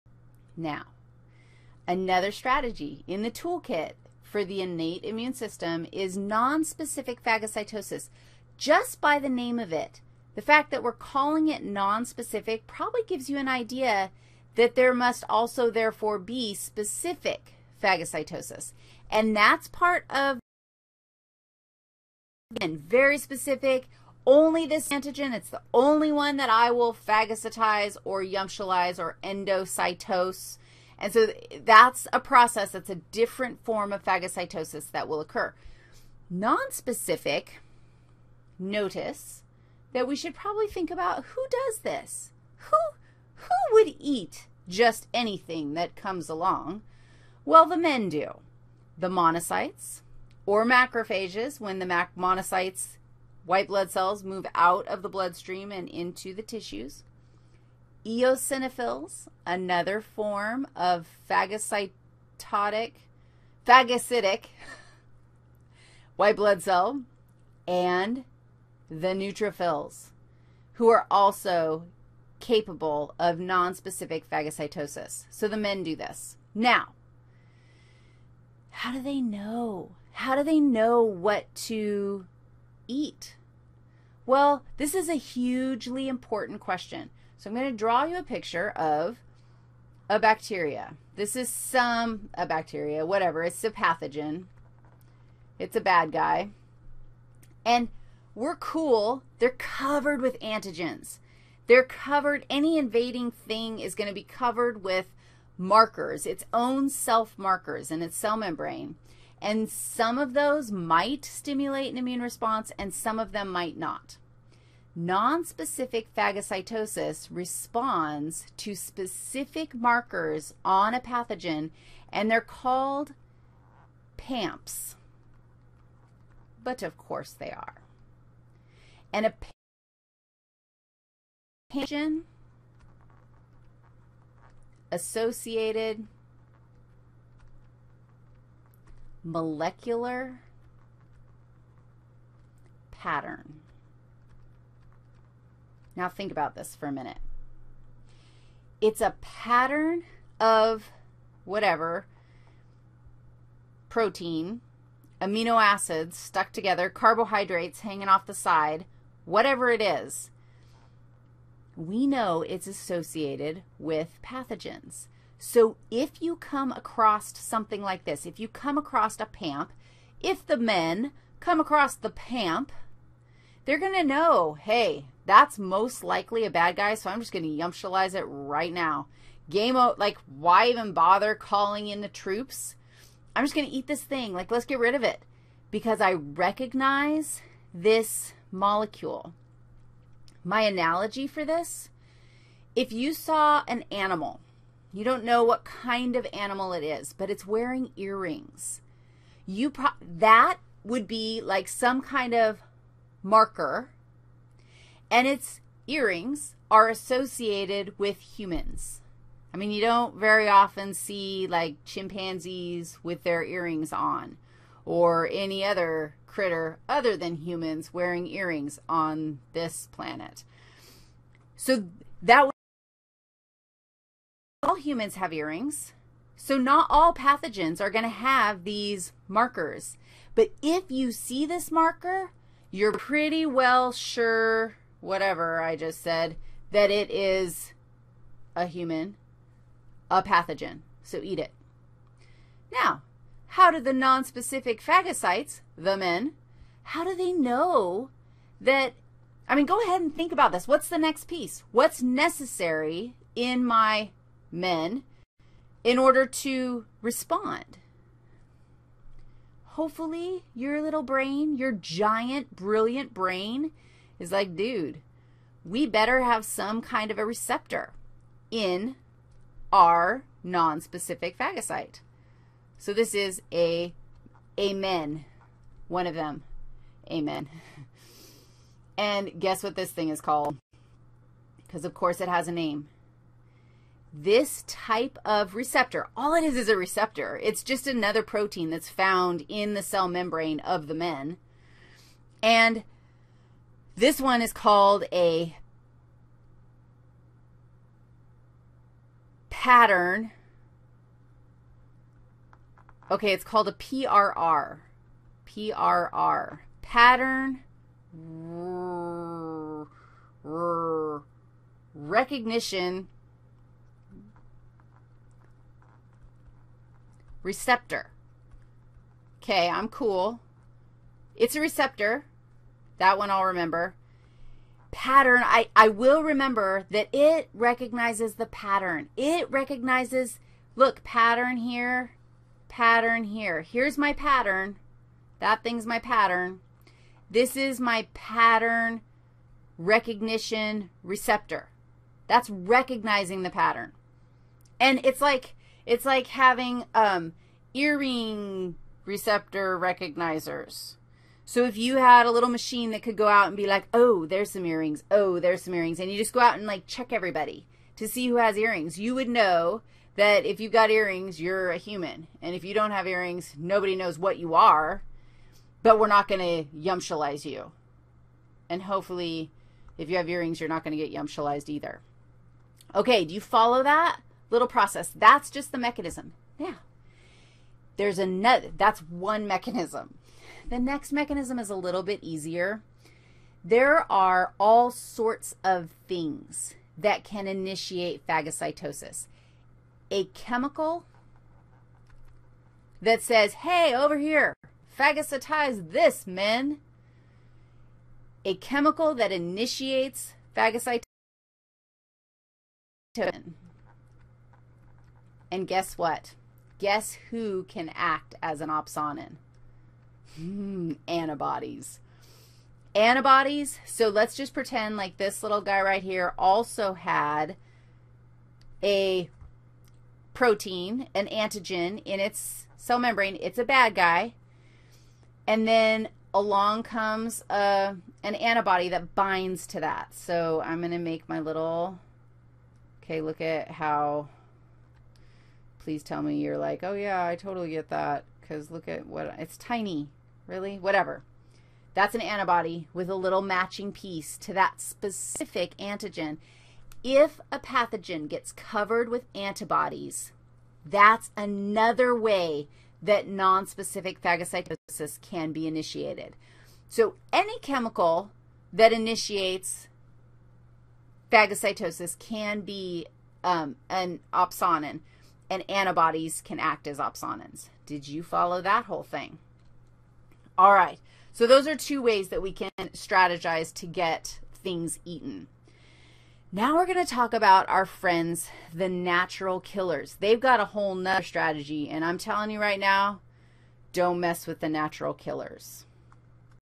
Innate Immunity 6- Nonspecific phagocytosis
Part 6 in a 8 part lecture on INNATE IMMUNITY in a flipped Human Physiology course taught by Wendy Riggs. CC-BY. Watch the whole lecture (all 8 videos) by going to the PLAYLIST